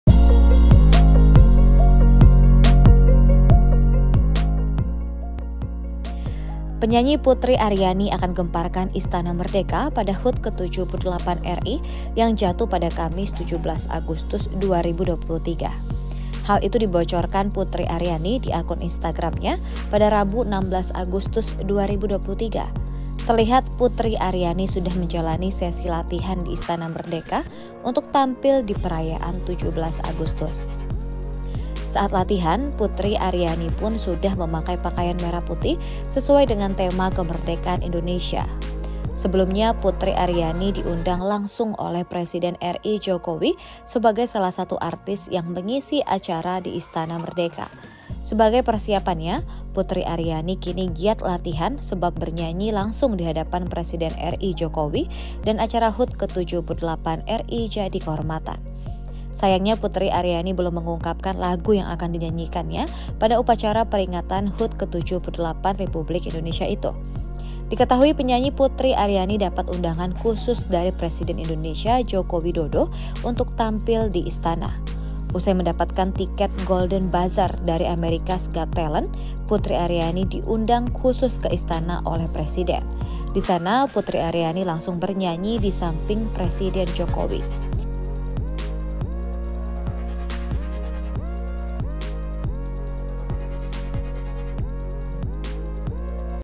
Diundang Langsung Presiden, Putri Ariani Siap Gemparkan Istana Di Perayaan HUT ke-78 RI
.Penyanyi Putri Ariani akan gemparkan Istana Merdeka pada HUT ke-78 RI yang jatuh Kamis (17/8/2023).

Hal itu dibocorkan Putri Ariani di akun instagramnya pada Rabu (16/8/2023).

Terlihat Putri Ariani sudah menjalani sesi latihan di Istana Merdeka untuk tampil di perayaan 17 Agustus.

Saat latihan, Putri Ariani pun sudah memakai pakaian merah putih sesuai dengan tema Kemerdekaan Indonesia.

Sebelumnya Putri Ariani diundang langsung oleh Presiden RI Jokowi sebagai salah satu artis yang isi acara di Istana Merdeka.

vp rizky wardianti